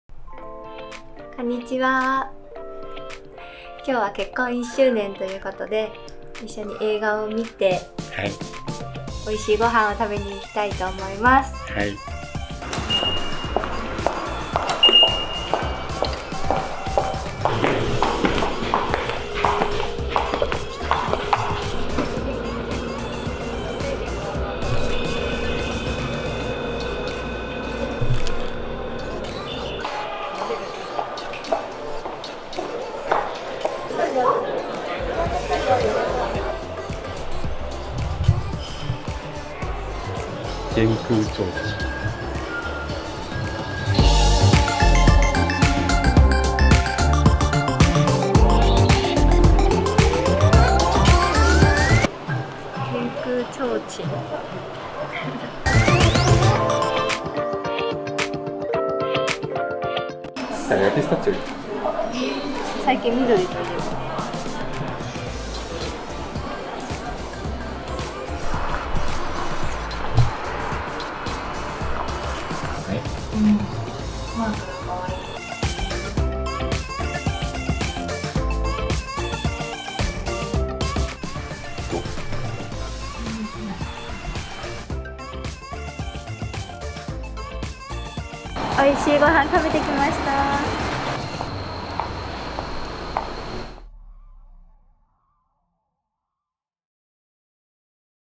【Vlog】結婚1周年
つい先日、結婚1周年を迎えることができました。
結婚1周年記念に気になっていたお店に
ディナーを食べにいってきました。
すごくおいしい料理で大満足でした。